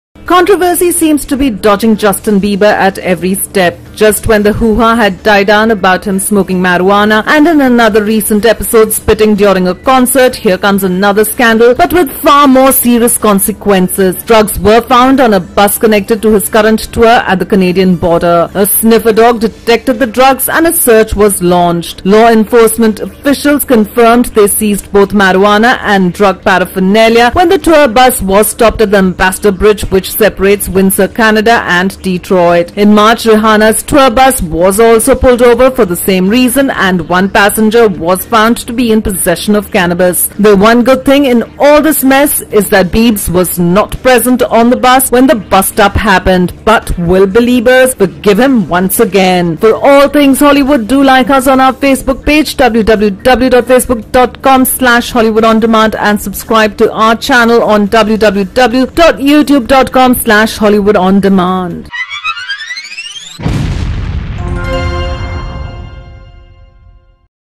SHOCKING Justin Bieber's Tour Bus Busted For Drugs
DETROIT — U.S. border agents found marijuana on a bus with singer Justin Bieber's tour as it crossed into Detroit from Windsor, Canada.

The Detroit Free Press reports  that U.S. Customs and Border Protection spokesman Ken Hammond confirms that the bus was stopped Sunday as it attempted to enter the U.S. on the Ambassador Bridge.

The singer was not on the bus at the time and performed later that night at Joe Louis Arena.

Hammond says a police dog indicated the presence of drugs on the bus and that drug paraphernalia and a small amount of marijuana were found. He says the bus driver was cited and that the bus and its passengers were allowed to go.